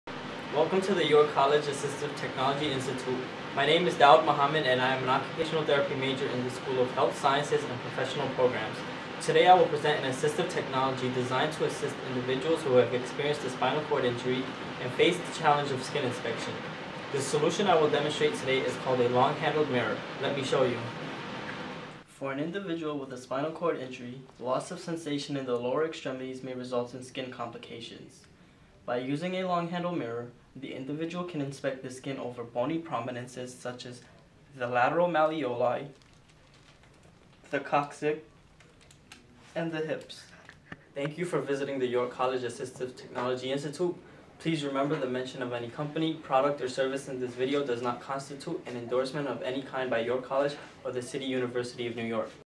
Long Handle Mirror- YCATI
This assistive technology may be used by individuals who need to inspect their skin in locations that are not easily visible.